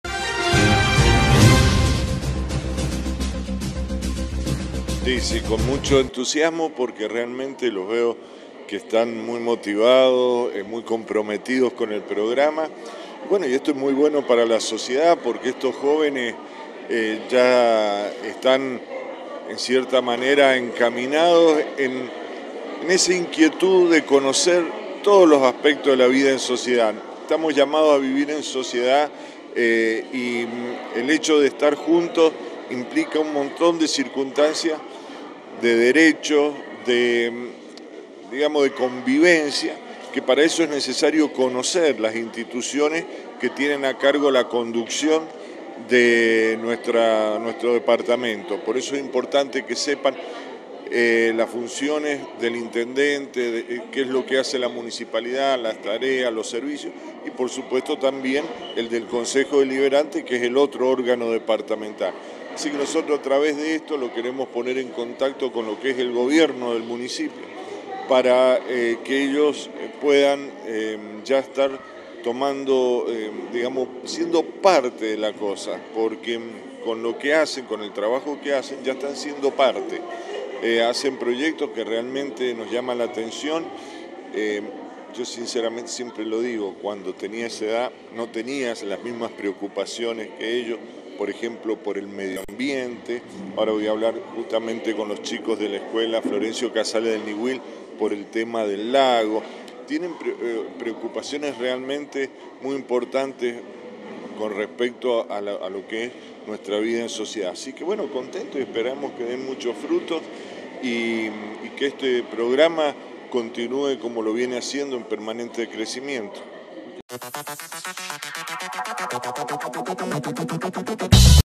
UNA SOCIEDAD QUE SE POLITIZA
El Presidente del HCD sanrafaelino, Ctdor. Ricardo Vergara expresó su entusiasmo y el compromiso de los estudiantes que comenzaron a participar del "Concejo Deliberante Estudiantil".